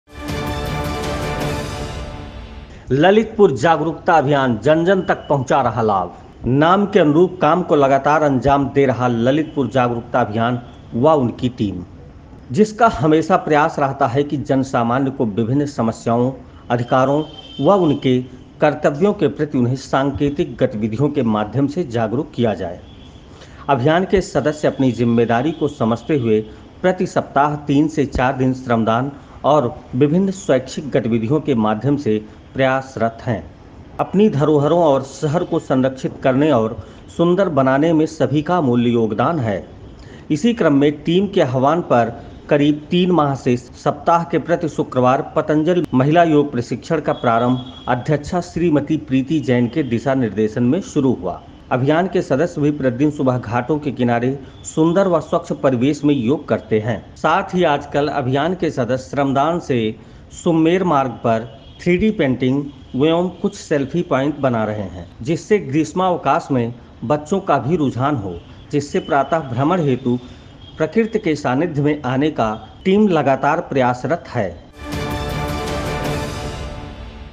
ललितपुर जागरूकता अभियान
ललितपुर - जनसामान्य की विभिन्न समस्याओं,अधिकारों और कर्तव्यों के प्रति लोगों को सांकेतिक गतिविधियों के माध्यम से जागरूक किया जाए। अभियान के सदस्य अपनी इस जिम्मेदारी को समझते हुए प्रति सप्ताह तीन से चार दिन श्रमदान और विभिन्न स्वैच्छिक गतिविधियों के माध्यम से प्रयासरत हैं,अपनी धरोहरों और शहर को संरक्षित करने और सुंदर बनाने में इसी क्रम में टीम के आव्हान पर करीब तीन माह से सप्ताह के प्रति शुक्रवार पतंजलि महिला योग प्रशिक्षण की अध्यक्षा श्रीमती प्रीति जैन की उपस्थिति में निःशुल्क योग प्रशिक्षण भी दिया जाता है